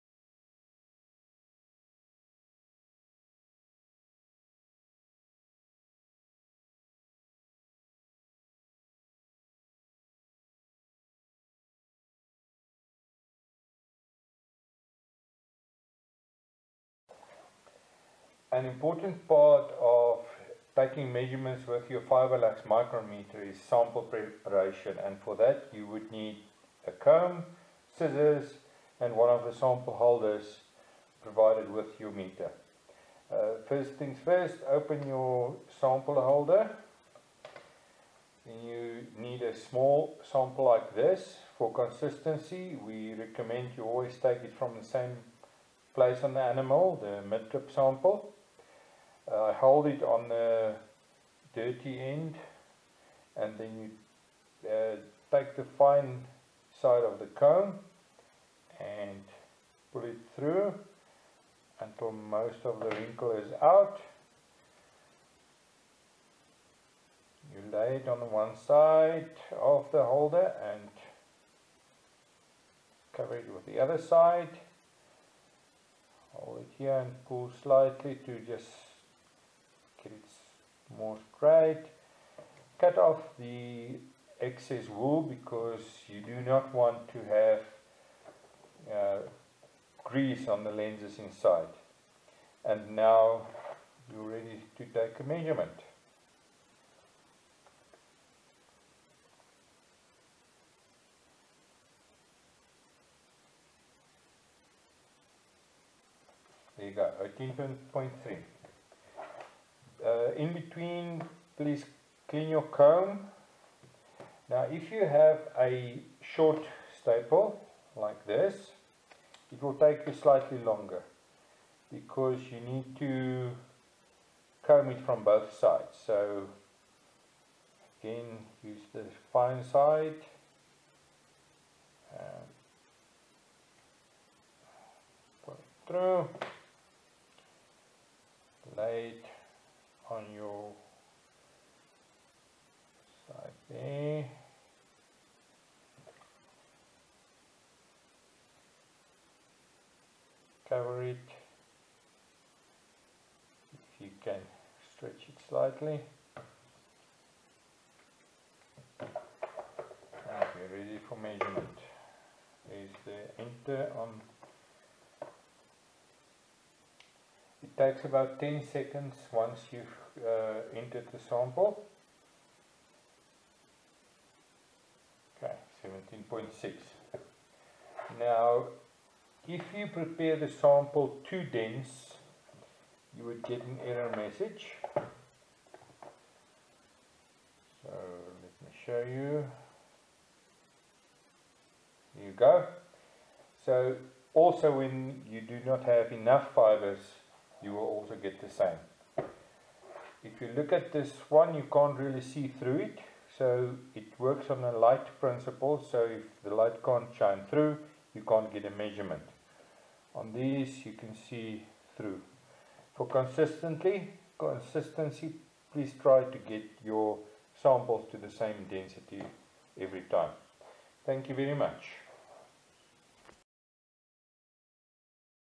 Fibrelux Sample Preparation - Wool & Huacaya Alpaca
The FibreLux Micron Meter is a hand held unit that can be used to obtain immediate and accurate fiber diameter readings. It can be used on shearing day to assist in sorting, increasing potential  revenue from fiber sales. It can be used to assist in herd evaluation. The Fibrelux  can also improve the quality of decisions made when considering purchase of  Alpacas or other fiber stock.